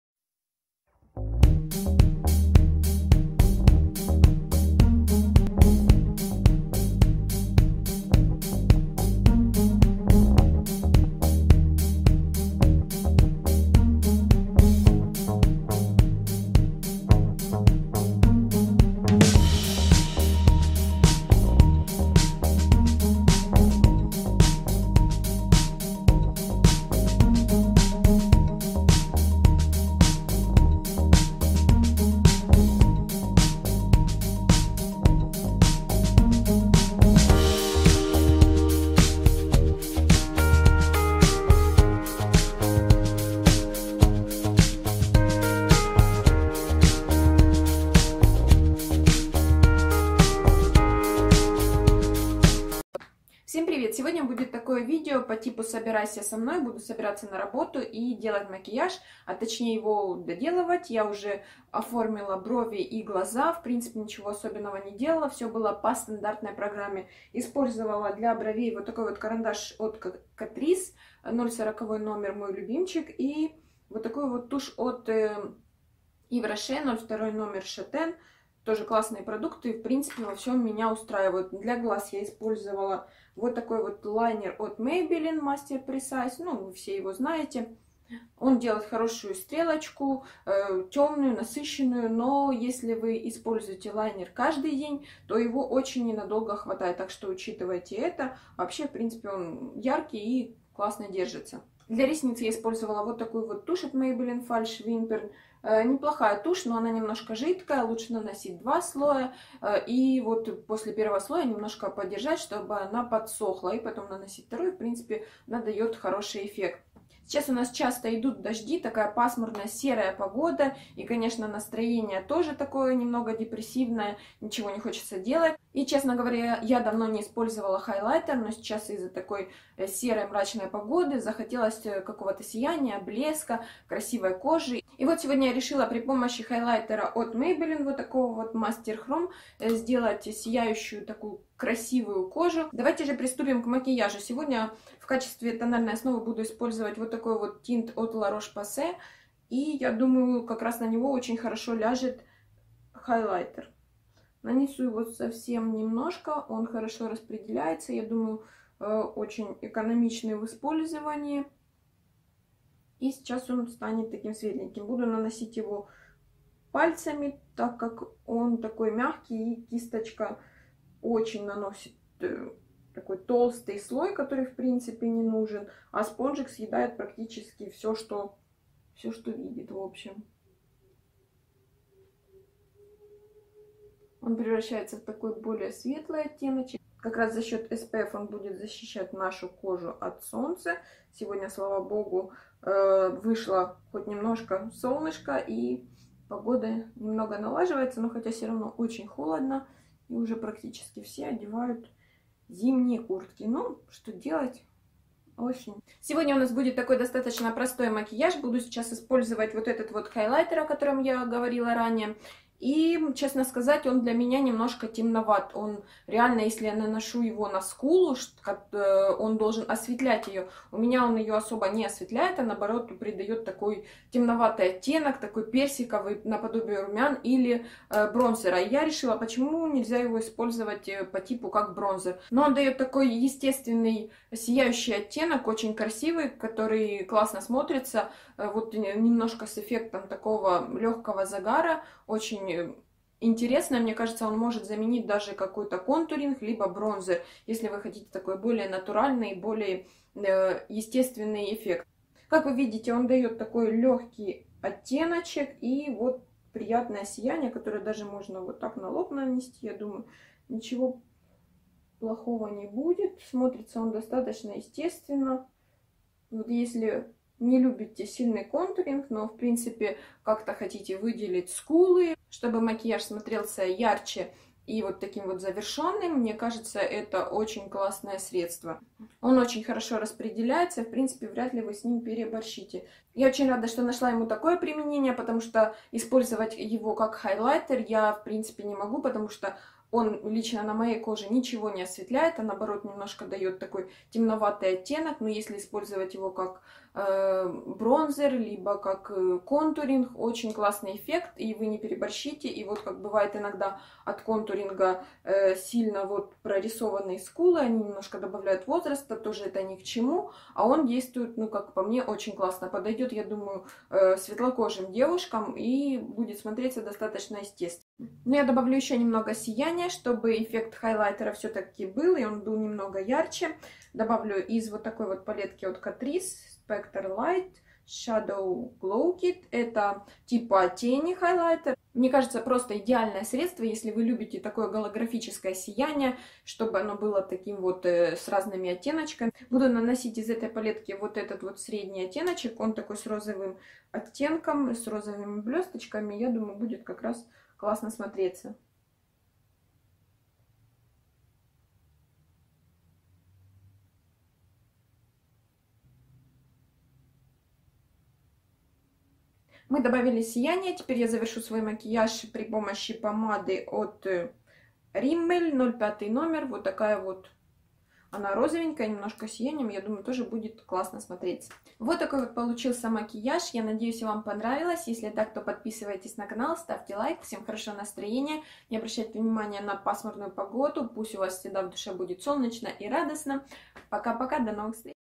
Собирайся со мной/Get ready with me/
Косметика использованная в макияже:
La Roche Posay toleriante teint 01 ivory;
Тушь для бровей от Yves Rocher 02;
Maybelline The Falsche Wimpern;
Maybelline master precise liquid eyeliner;
Catrice eye brow 040;
Rimmel 05;
Maybelline master Chrome 050;
Catrice spectra light eyeshadow glow kit 010